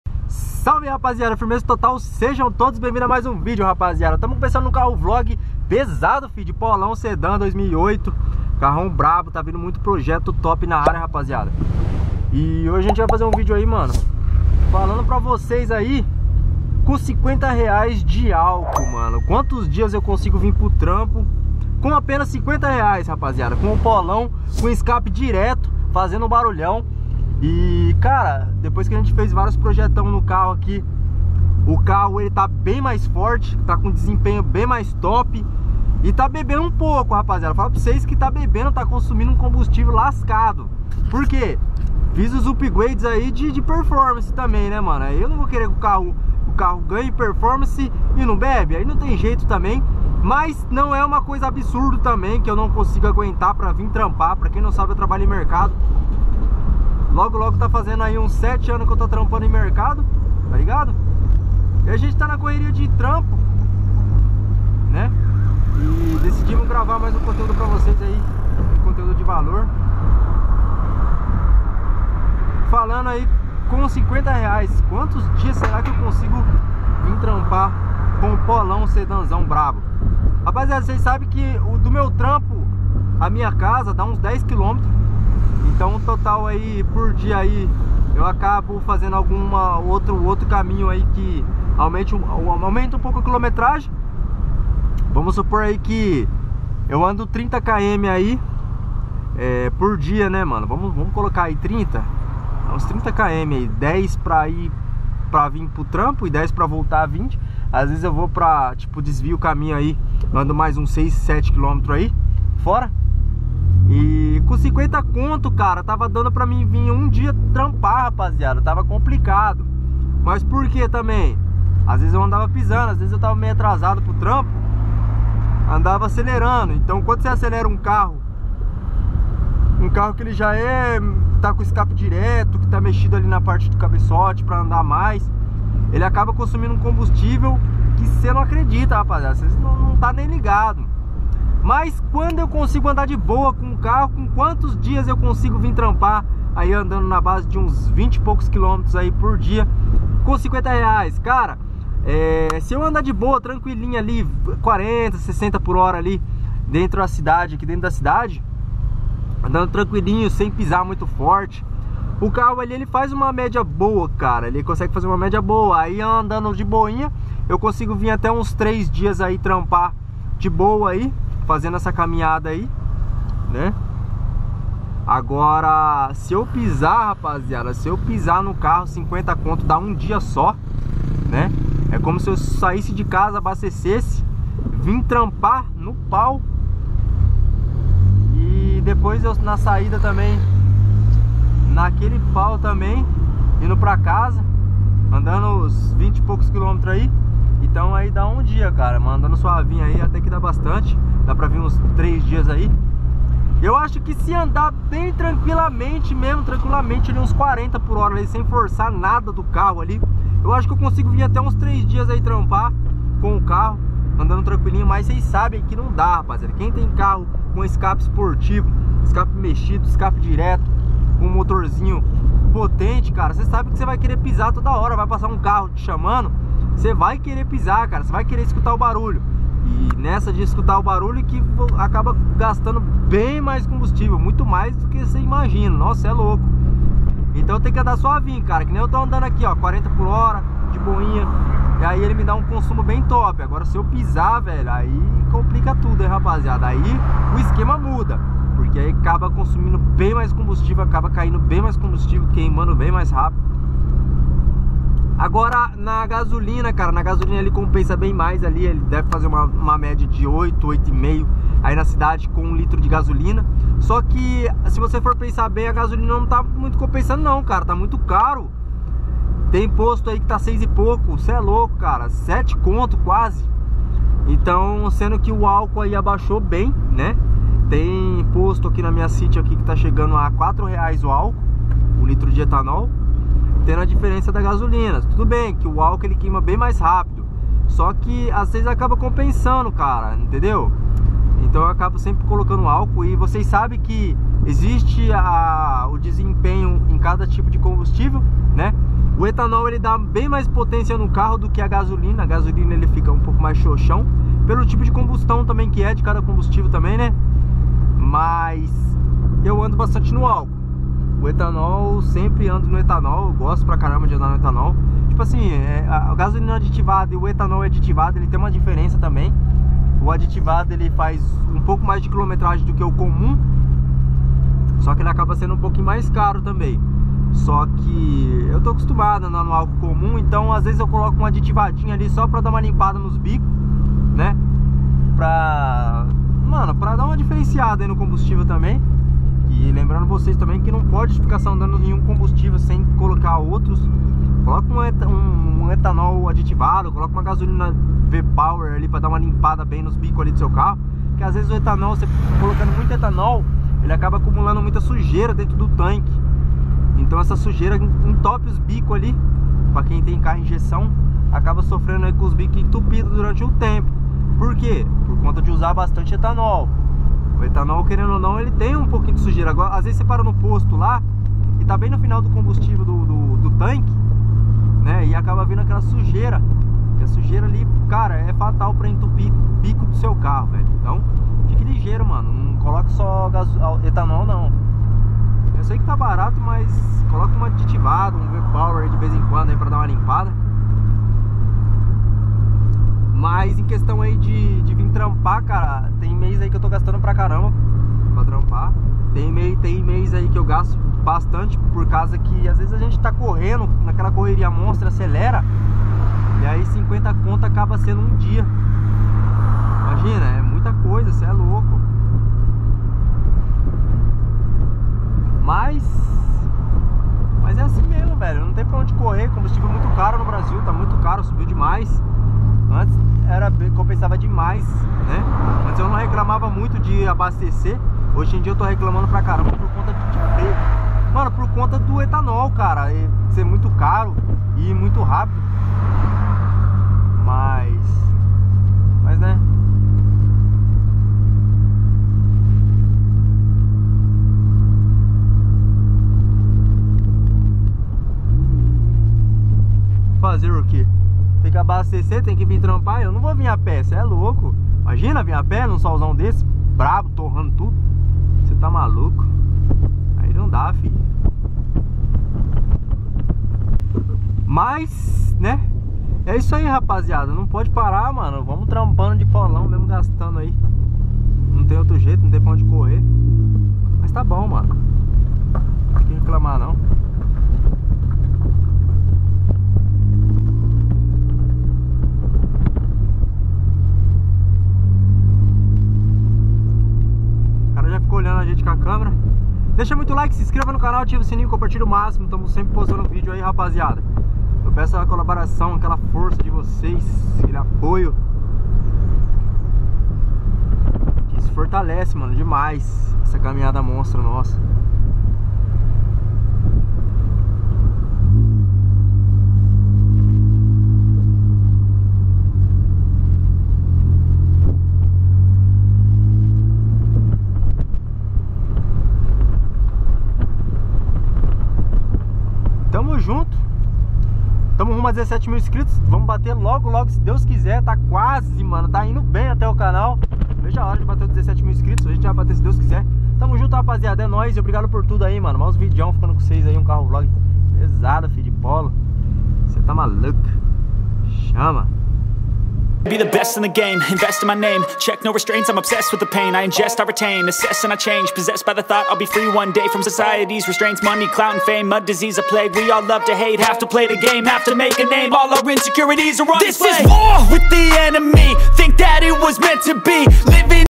Quantos Dias Consigo Trabalhar Com 50 Reais De Combustivel No Polo Com Escape Direto
salve galera mais um video novo no canal e dessa vez falando um pouco sobre o consumo do polo , e quanto eu consigo ir trampar de polo com apenas 50 reais acho que muitos devem ter essa curiosidade , entao assistam ajudem o canal COMPARTILHEM PRA 10 AMIGOS ...

► Carro : POLO SEDAN 2008
► Carro : VOYAGE QUADRADO ANO 85
► Carro : FIESTA SUPER CHARGE ANO 2003